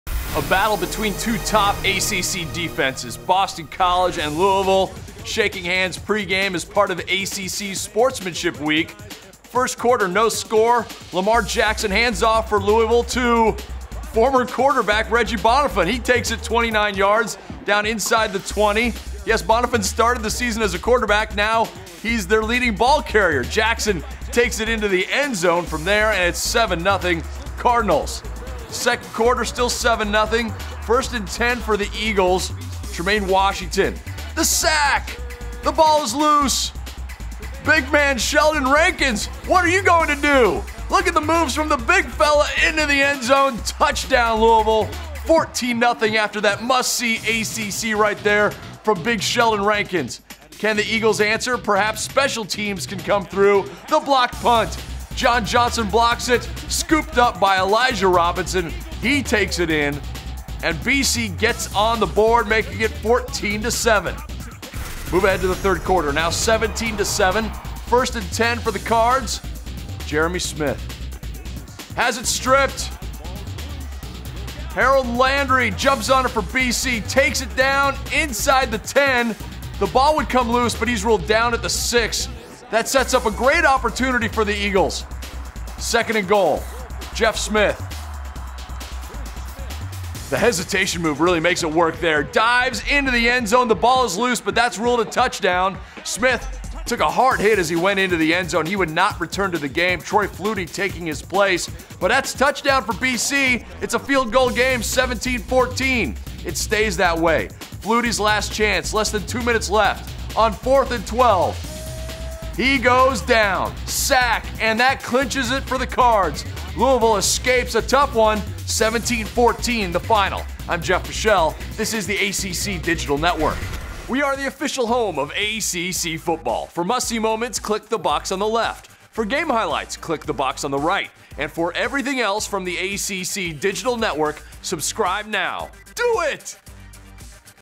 Boston College vs. Louisville Football Highlights (2015)
Boston College put up a hard fight against Louisville Saturday, but the Cardinals came away with the 17-14 win.  The Cardinals piled up 8 sacks on the day with one leading to a fumble that was returned for a key touchdown by Sheldon Rankins.  The Cards hit the road next week to take on Wake Forest in a Friday night tilt.